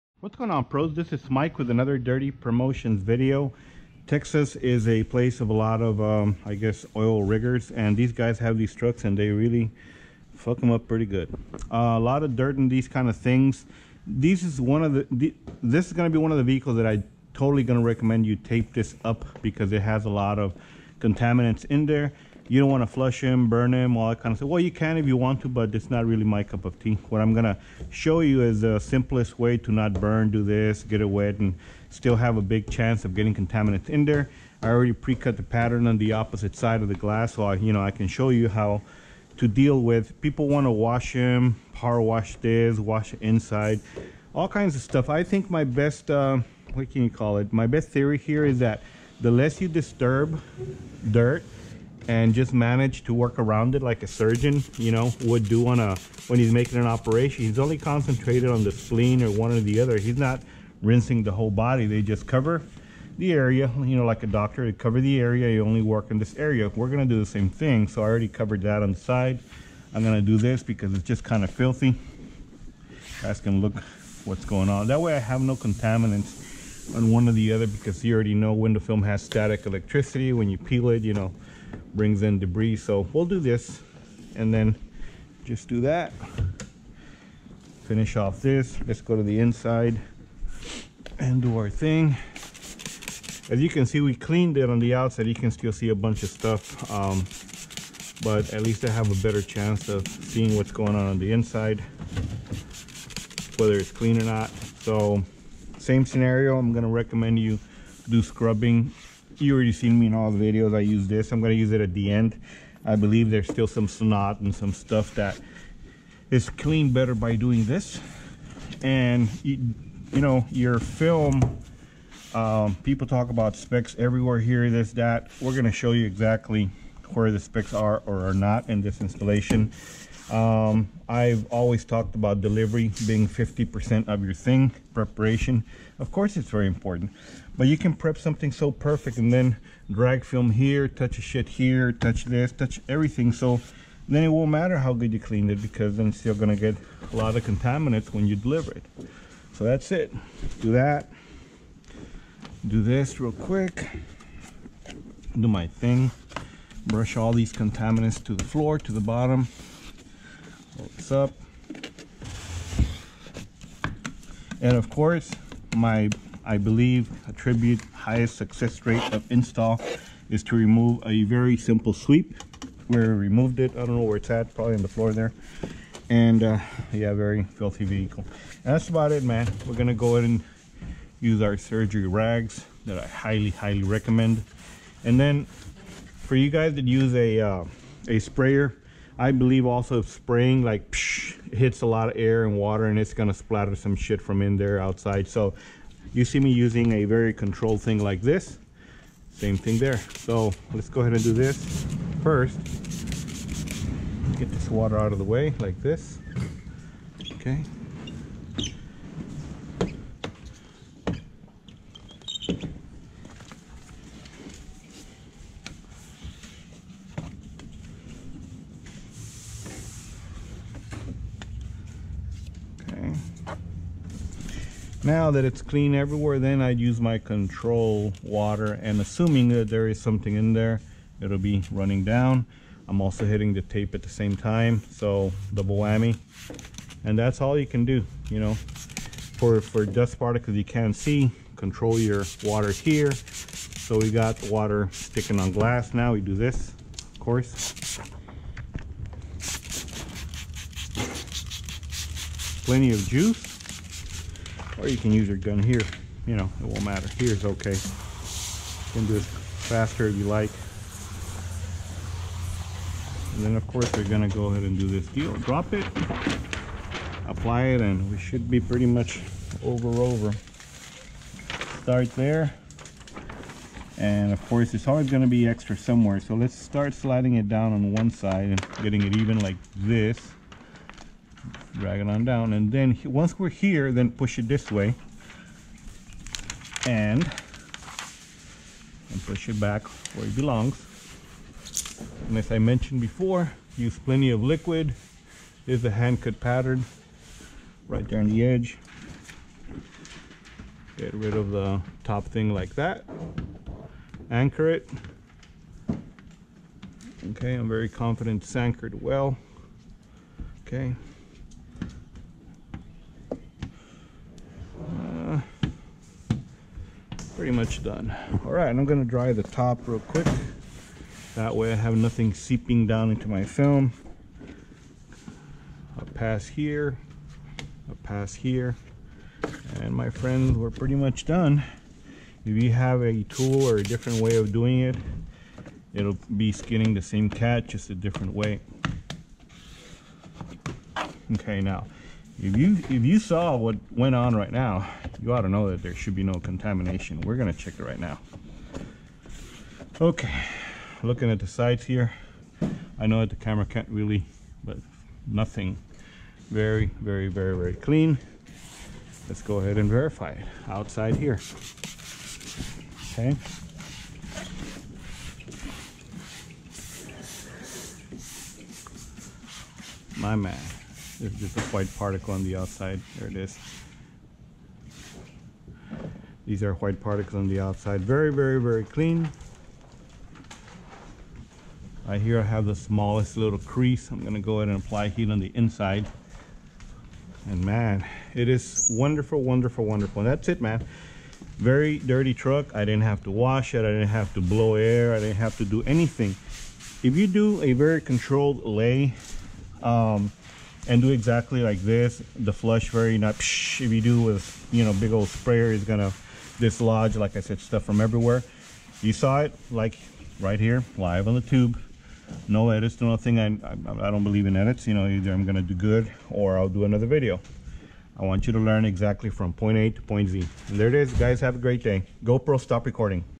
Window Tint Installation Surrounded by Tons of DIRT!!! ***Do Not Miss!!****
As i have always preached. cleaning is 50% of ur install success...Delivery of Film is the other 50%!!! Please share and tag a friend who might find this Useful!!! dirtypromotions dirty-tools.com Enjoy window tint tinting